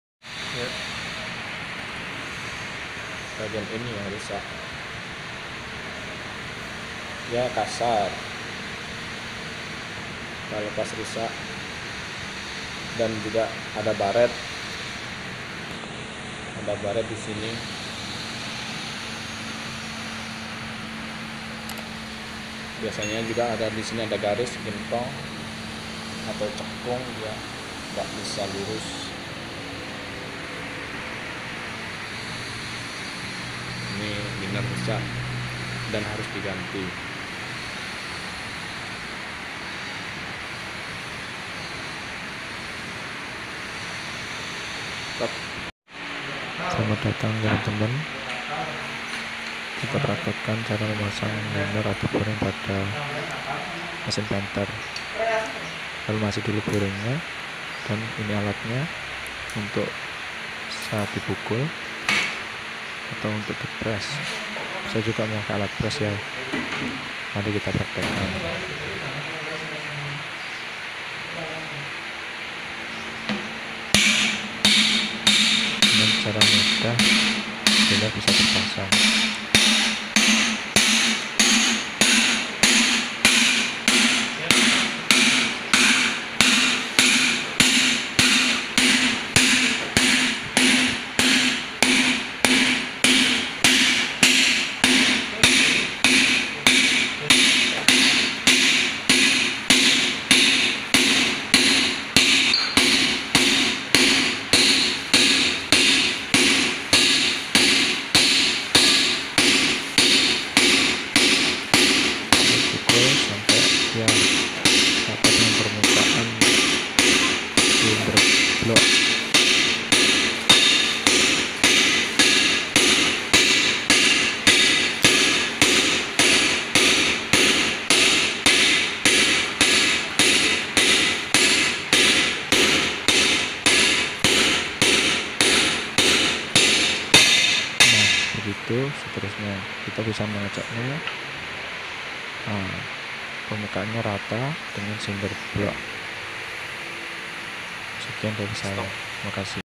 cara pasang boring/liner panther
cara manual pasang boring atau liner pada mesin panther